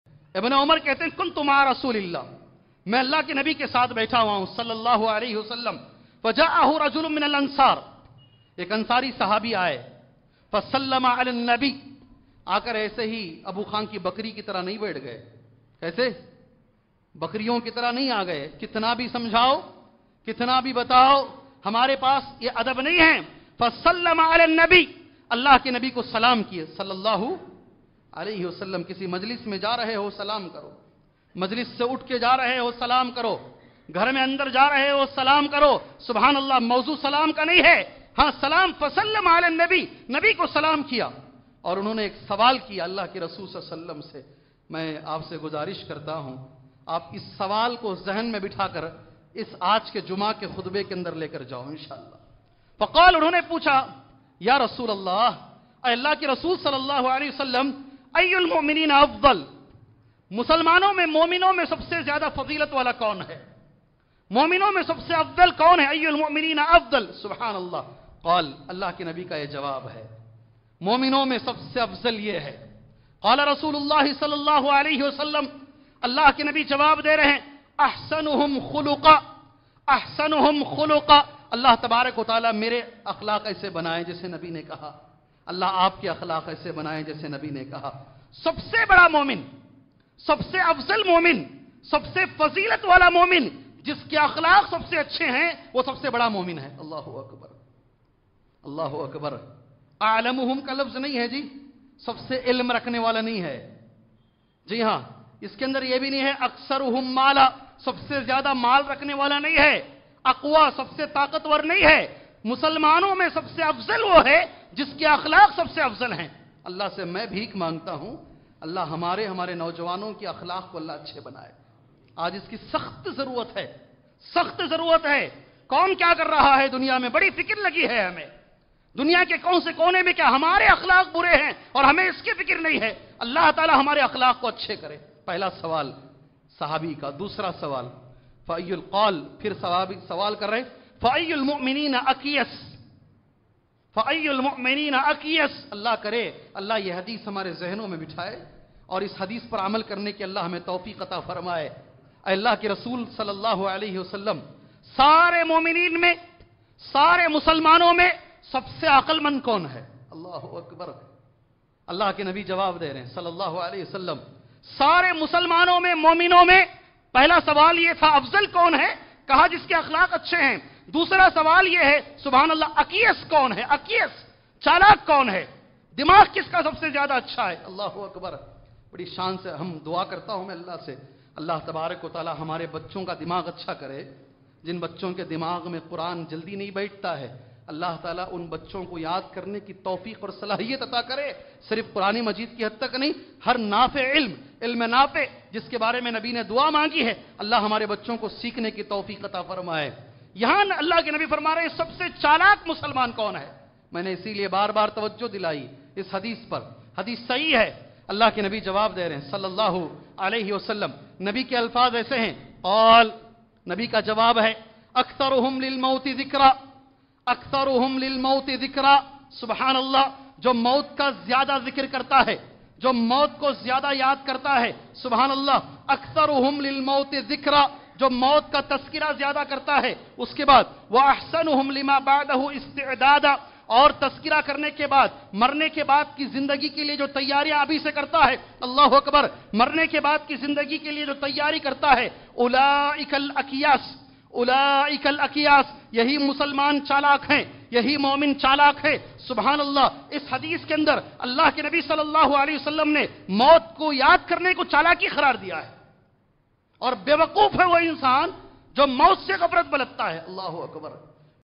Aqalmand Momin | Shaykh Abdul Qadeer Umri
Topic: Aqalmand Momin

This clip is taken from the lecture:
Dunyavi Zindagi Ki Asal Haqeeqat

٠٠٠٠٠٠٠٠٠٠٠٠٠٠٠٠٠٠٠٠٠٠٠٠٠٠٠٠٠٠٠٠٠٠٠٠٠٠٠٠